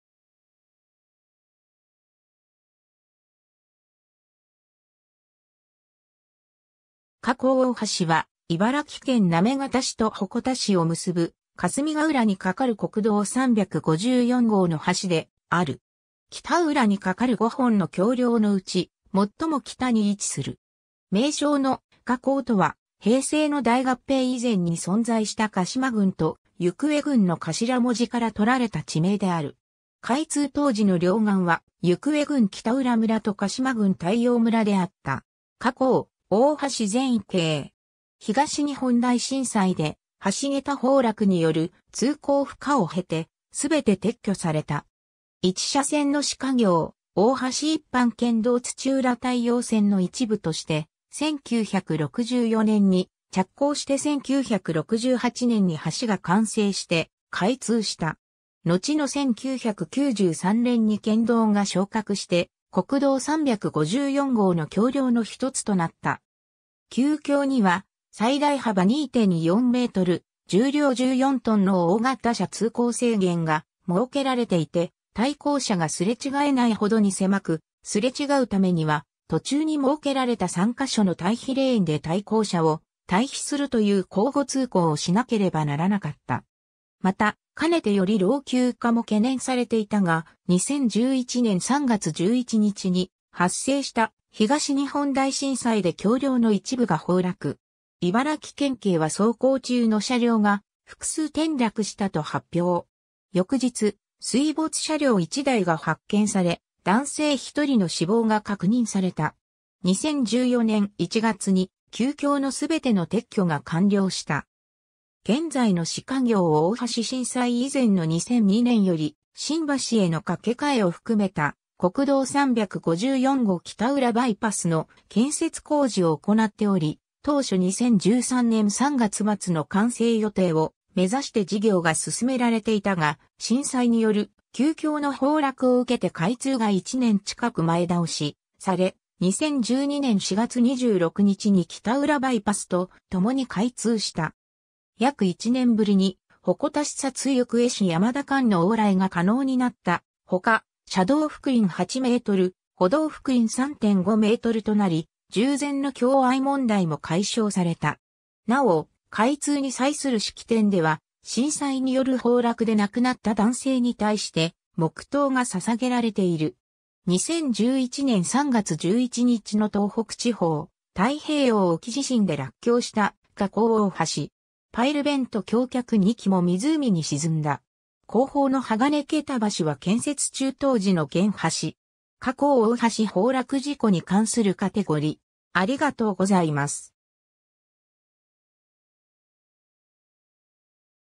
鹿行大橋
鹿行大橋（ろっこうおおはし）は、茨城県行方市と鉾田市を結ぶ、霞ヶ浦（北浦）に架かる国道354号の橋である。
 北浦に架かる5本の橋梁のうち、最も北に位置する。
 名称の“鹿行”とは、平成の大合併以前に存在した鹿島郡と行方郡の頭文字から取られた地名である。
開通当時の両岸は行方郡北浦村と鹿島郡大洋村であった。
 鹿行大橋（旧橋） 全景。
東日本大震災で橋桁崩落による通行不可を経て、すべて撤去された。
（2008年6月） 1車線の鹿行大橋（旧橋） （2008年6月） 一般県道土浦大洋線（当時）の一部として、1964年に着工して1968年に橋が完成して開通した。
のちの1993年に県道が昇格して、国道354号の橋梁のひとつとなった。
 旧橋には「最大幅2.4m、重量14t」の大型車通行制限が設けられていて、対向車がすれ違えないほどに狭く、すれ違うためには、途中に設けられた3ヶ所の待避レーンで対向車を待避するという交互通行をしなければならなかった。
 また、かねてより老朽化も懸念されていたが、2011年3月11日に発生した東日本大震災で橋梁の一部（行方市側より7〜9桁目の3径間、約58 m）が崩落。
茨城県警は走行中の車両が複数転落したと発表。
翌日、水没車両1台が発見され、男性1人の死亡が確認された。
2014年1月に旧橋のすべての撤去が完了した。
 現在の鹿行大橋（2代目） （2017年11月） 震災以前の2002年より新橋への架け替えを含めた国道354号北浦バイパスの建設工事を行っており、当初2013年3月末の完成予定を目指して事業が進められていたが、震災による旧橋の崩落を受けて開通が1年近く前倒しされ、2012年4月26日に北浦バイパスと共に開通した。
約1年ぶりに鉾田市札-行方市山田間の往来が可能になったほか、車道幅員8 m（2車線）・歩道幅員3.5 mとなり従前の狭隘問題も解消された。
なお、開通に際する式典では、震災による崩落で亡くなった男性に対して黙祷が捧げられている。
 2011年3月11日の東北地方太平洋沖地震で落橋した鹿行大橋（旧橋）。
パイルベント橋脚2基も湖に沈んだ。
後方の鋼桁橋は建設中当時の現橋。
 鹿行大橋崩落事故に関するカテゴリ: